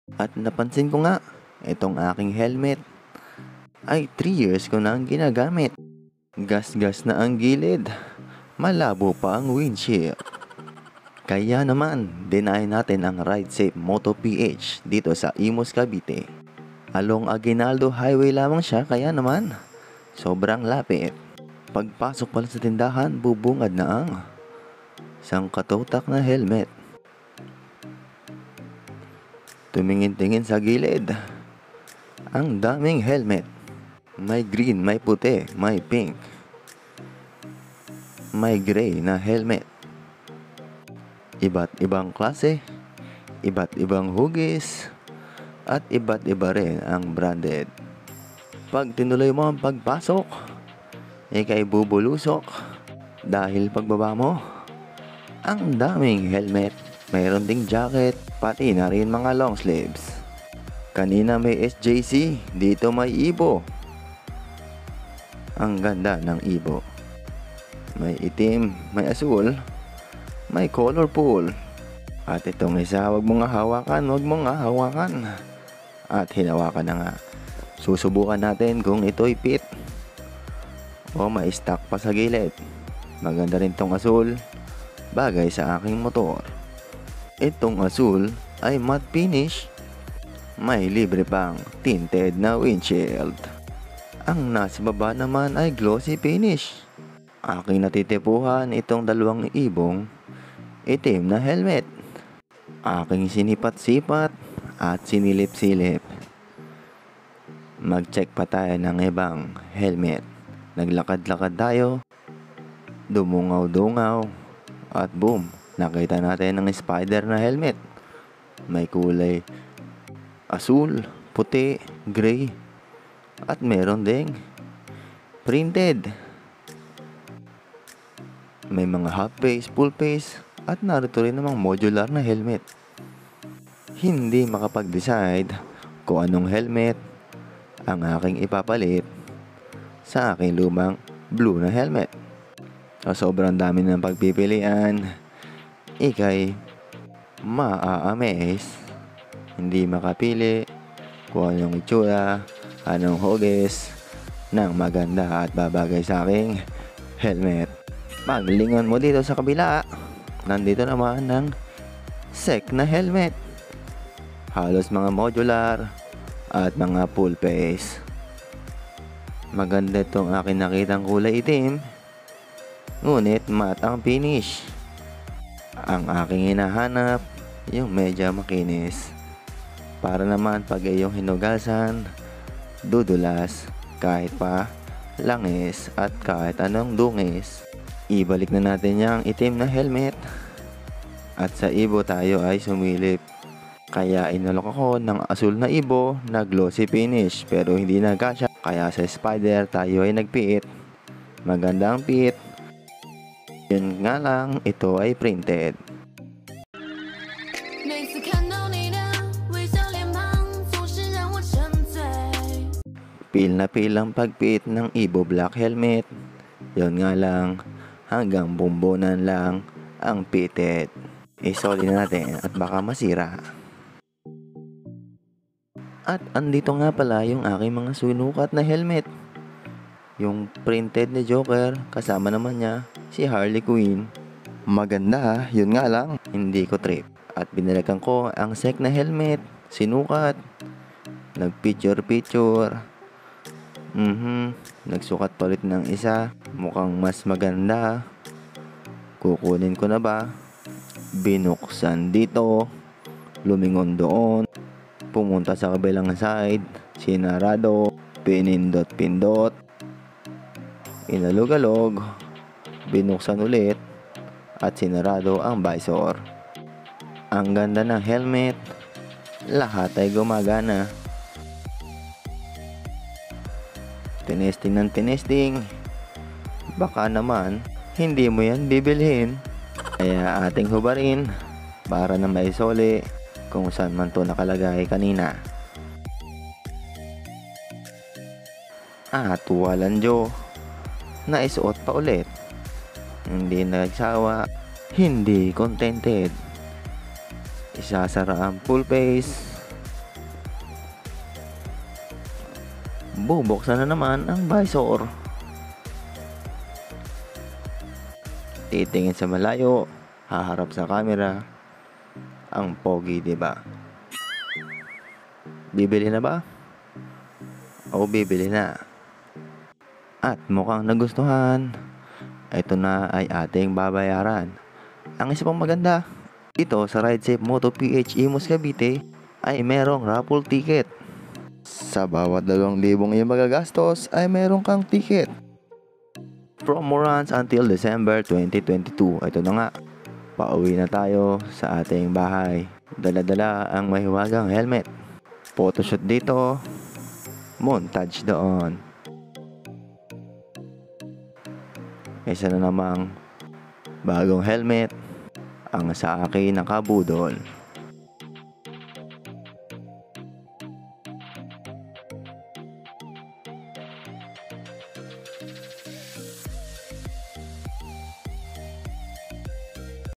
Gasgas na ang helmet | Ride safe moto Ph | Sen TeeVee
kaya naman minarapat na nating palitan ang di na gaanong safe na helmet, para ride safe!

Visit Ridesafe Moto Ph Imus Along aguinaldo Hi-way konteng kembot lang mula sa Robinsons Imus Cavite.